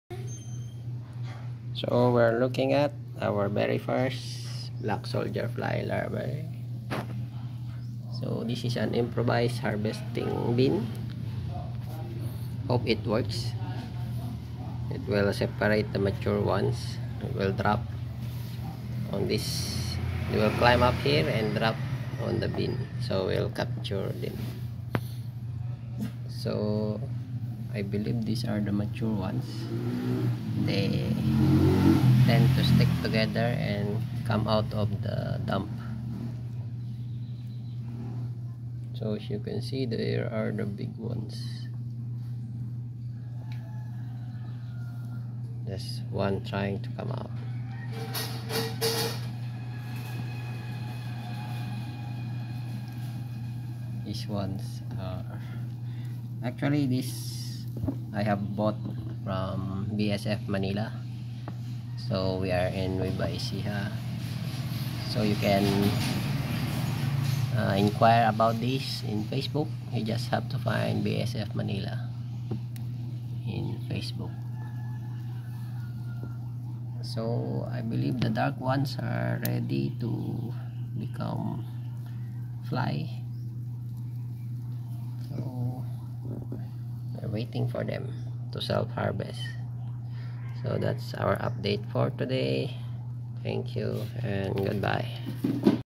BSF (black soldier fly larvae) improvised harvesting bin
Our very first bsf larvae acquired from 'bsf manila'. These will be the start-up colony of our bsf farm.